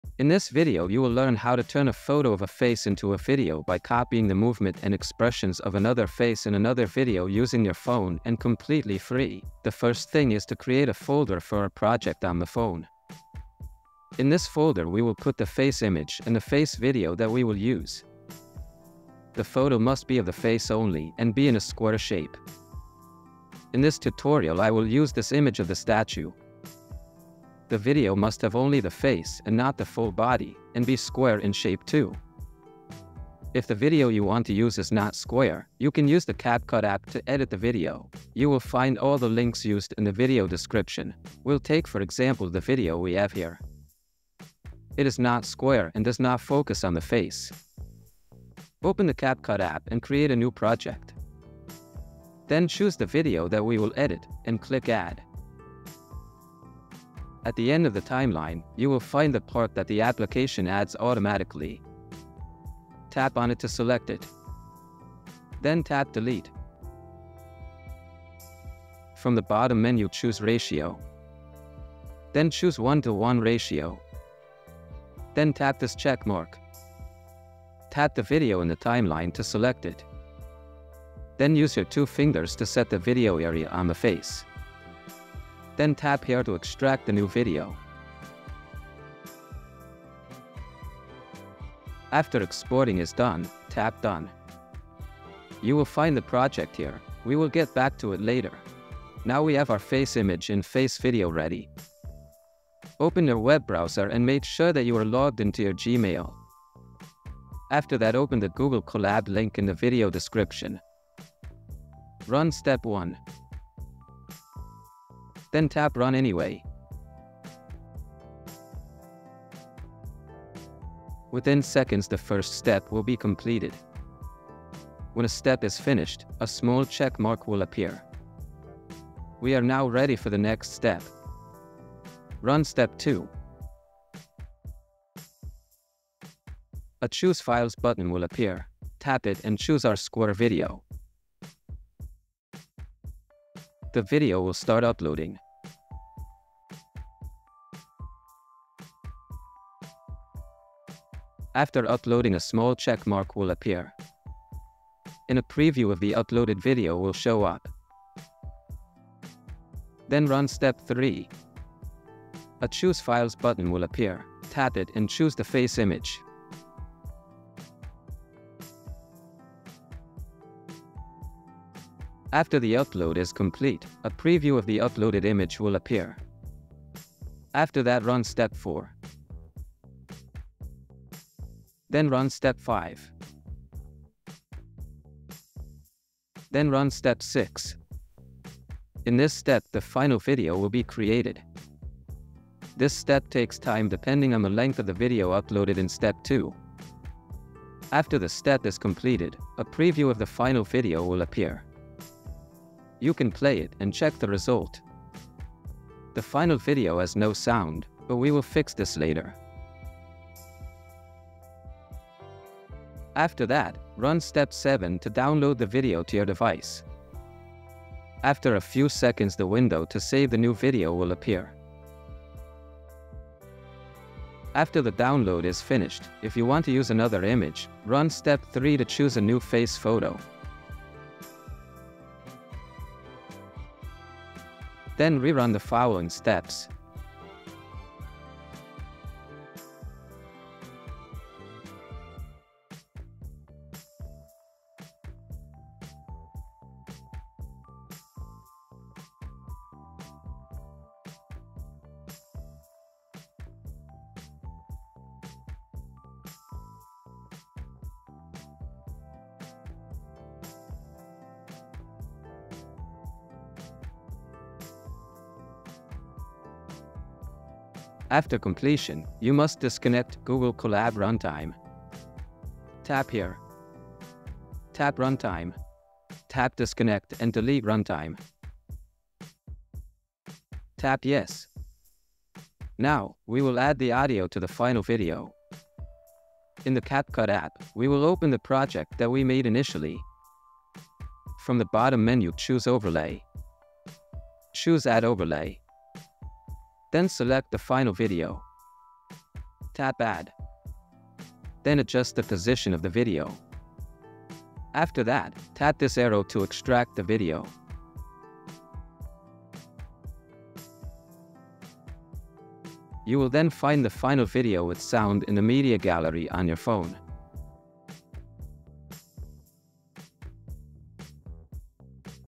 Photo To Video FREE - Colab (Thin Plate Spline Motion Model) Simplified
In this video you will learn how to animate face image and convert it to a talking or singing avatar using thin plate spline motion model on Google Colab for free.
You will learn an easy way to use thin plate spline motion model for image animation with simplified user interface. You can make any photo sing or speak using this easy AI tutorial.

✅ This channel "Bahaa AI" specializes in publishing tutorial for artificial intelligence tools to create videos, photos and music using a mobile phone.